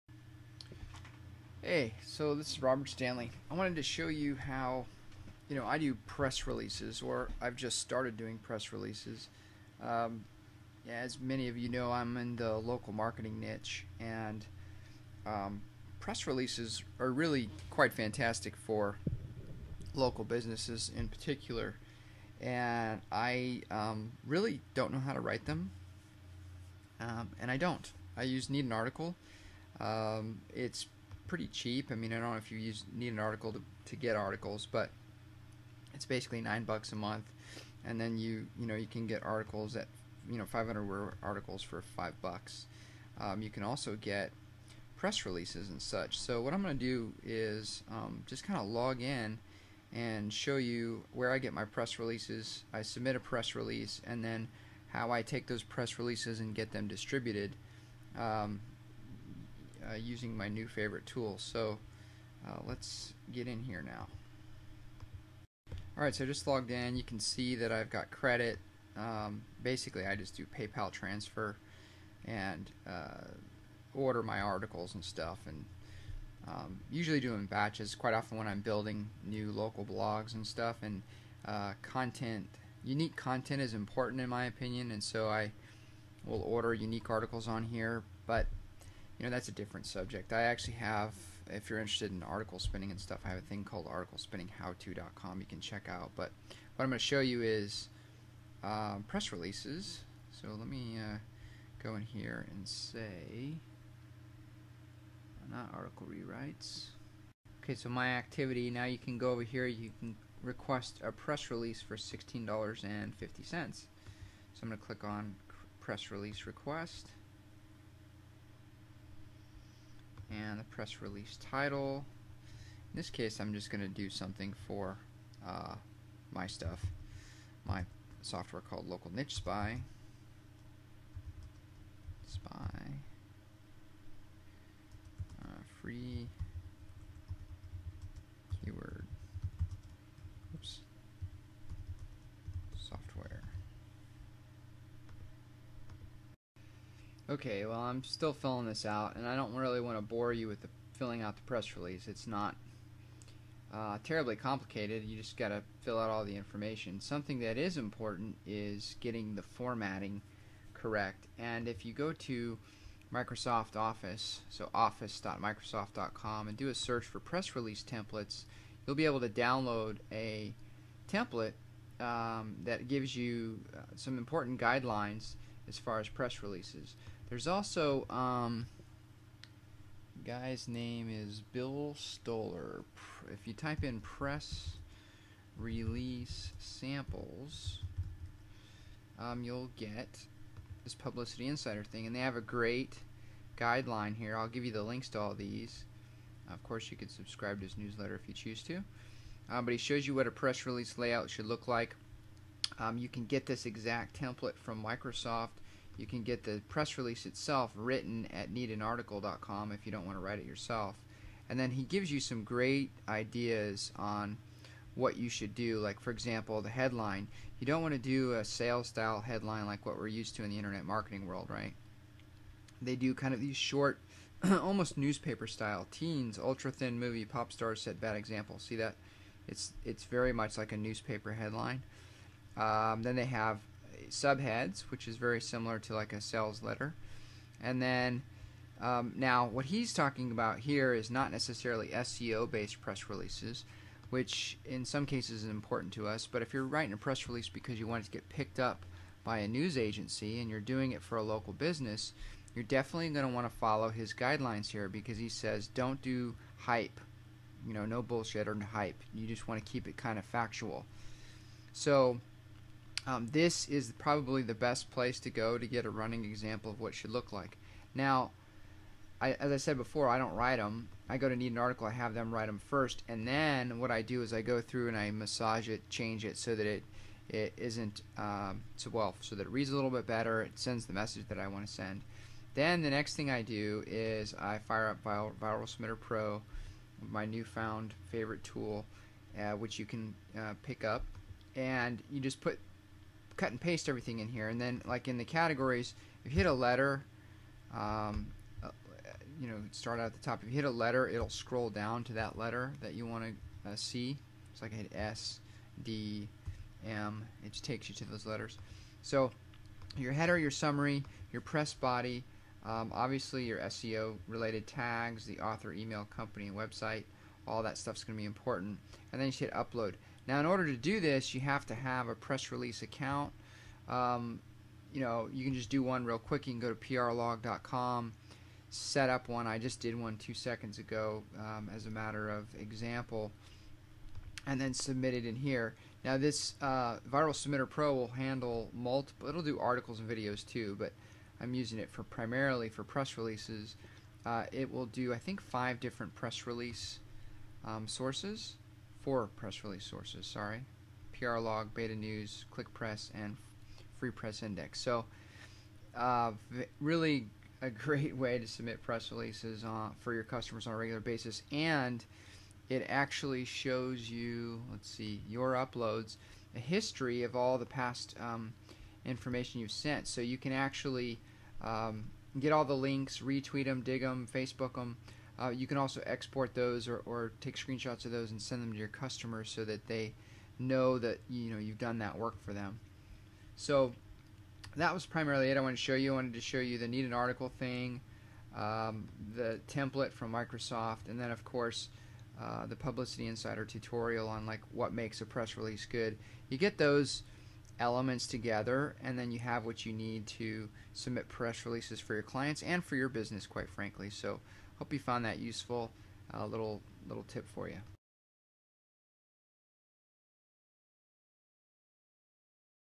Press Release Distribution Methods and Software for Local Business SEO
- Quickly create press releases and distribute them using this step-by-step easy to follow press release distribution example. Using this for your local business SEO is a "must have" search engine optimization strategy.